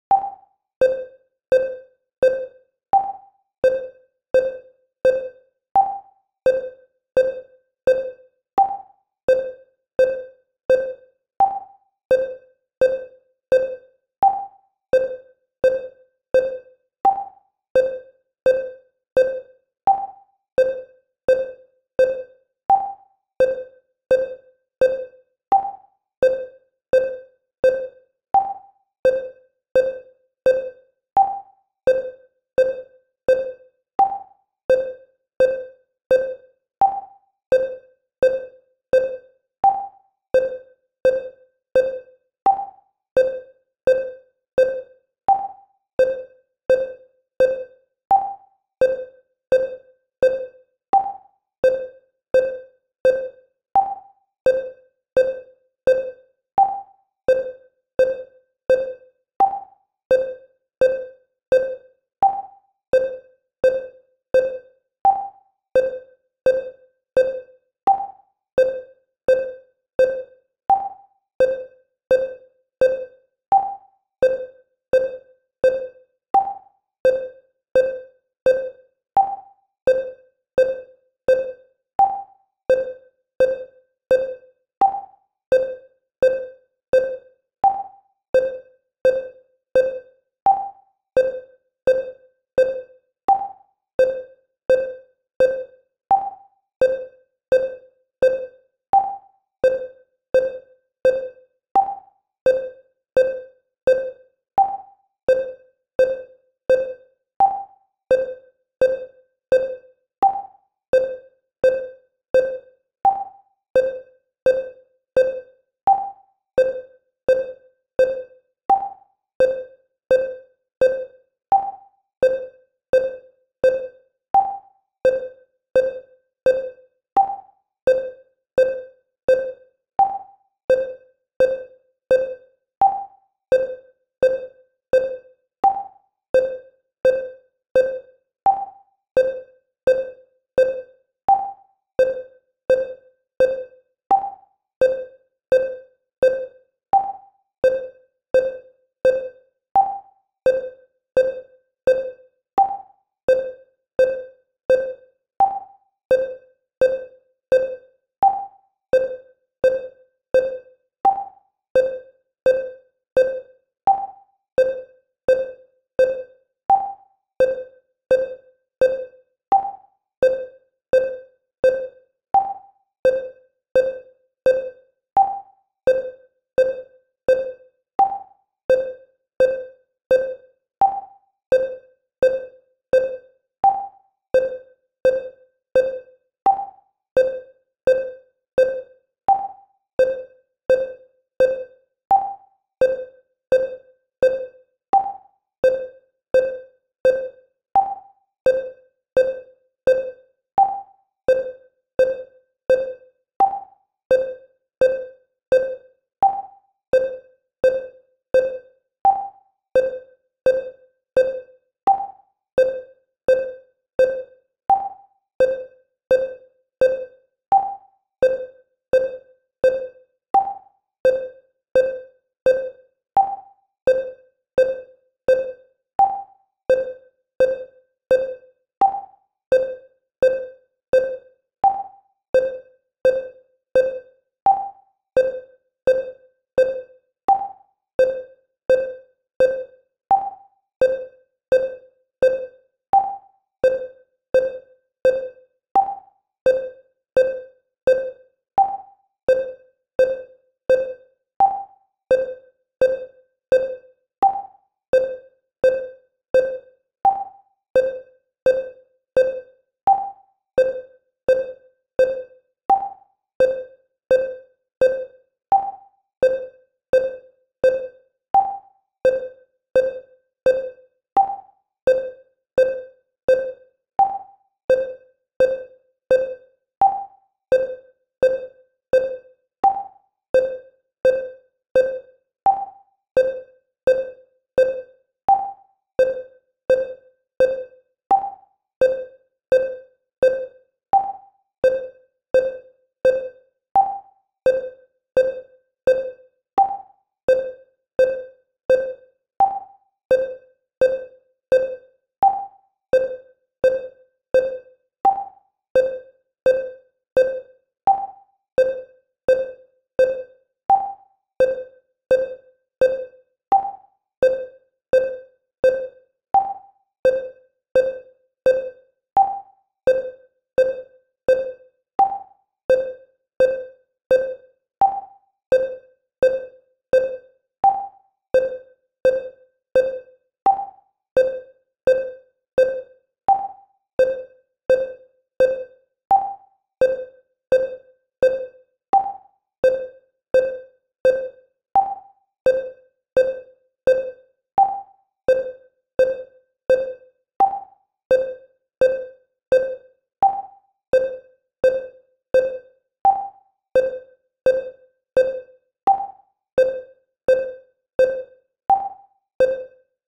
✅ 85 BPM (Beats Per Minute) Learn Fast, Practice Slow... Aprende Rápido, practica lento...
Practice 6 minutes a day your exercises with each level to improve.
Do you want a metronome that doesn´t get mixed with percussive instruments and has frequencies that you can hear even with everything playing?

Metronome Made in Mexico

I leave you here a series of Tempos from 50 to 200 to practice.

Práctica 6 minutos diarios tus ejercicios con cada nivel para ir subiendo,
Quieres un metrónomo que no se confunda con los instrumentos percusivos y tenga frecuencias que aún con todo sonando puedas escuchar?

Metrónomo Hecho en México

Te dejo aqui una serie de Tempos del 50 al 200 para practicar.

AbStudio